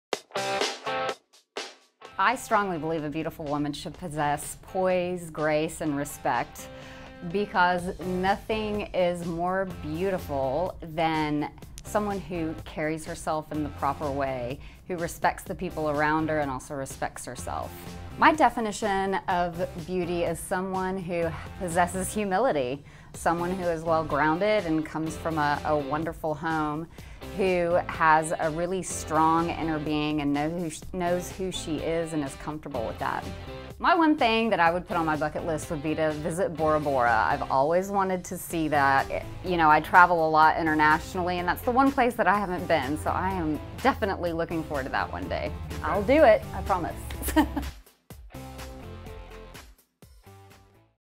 Kristi Beamish - 2012 Most Beautiful Women in Ft. Worth
Kristi Beamish has been nominated as a Top 20 Finalist for Fort Worth, Texas Magazine's Most Beautiful Women in Fort Worth.